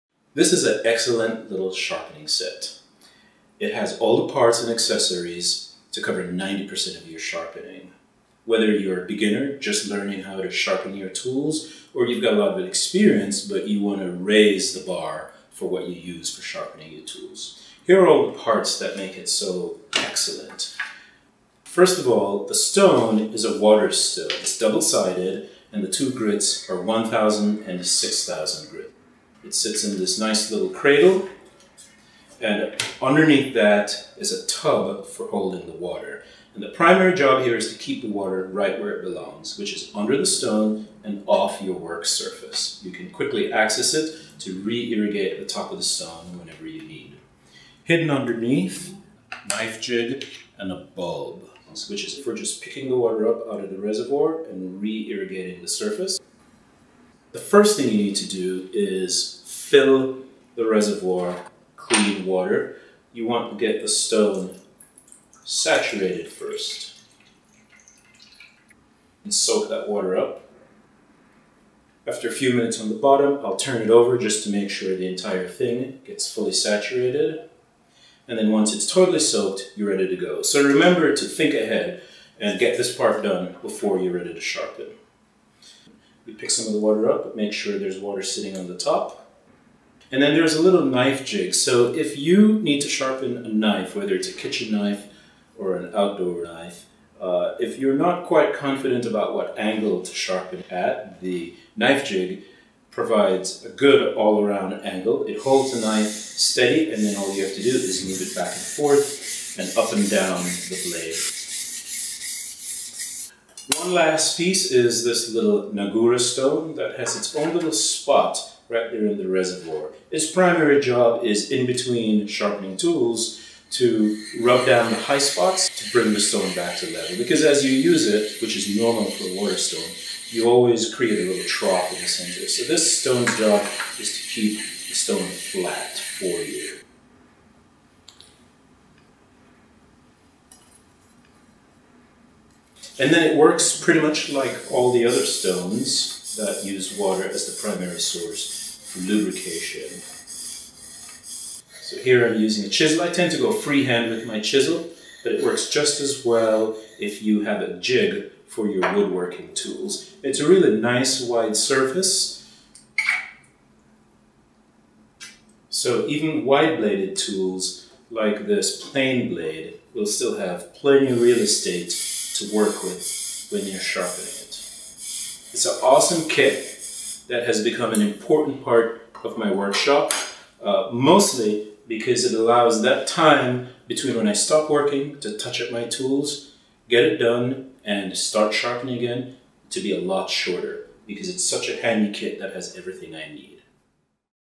5 Piece Sharpening Stone Set Garrett Wade
This Comprehensive Sharpening Set immediately caught our attention, because it has critical design features that make it a pleasure to use.
Start with the stone itself: a double-sided (1,000 and 6,000 grit) carborundum sharpening stone – sized an exceptionally large 3 x 8" (24 sq in.). A large surface like this is the very best for larger cutting tools, including long bladed knives.
The stone is held by by a special 2-way jig. In one configuration, the slope holds the stone level in the tray. This is for most typical usage. But for sharpening blades at especially low angles, turn the jig around and the stone will reposition at a low sloping angle, that gives your fingers clearance to work even lower.
The large plastic waterproof base, with its four stabilizing rubber feet, is designed to hold and contain the lubricating water plus the slurry produced during sharpening, that is so important in the action. Finally, there is the rubber squeeze ball (to squirt extra water on the surface); the clip-on guide to use with long blades (that help ensure a consistent bevel); and the small cleaning stone (used to clean off and dress the surface of the carborundum). This one is a gem. Very affordable. Highly recommended.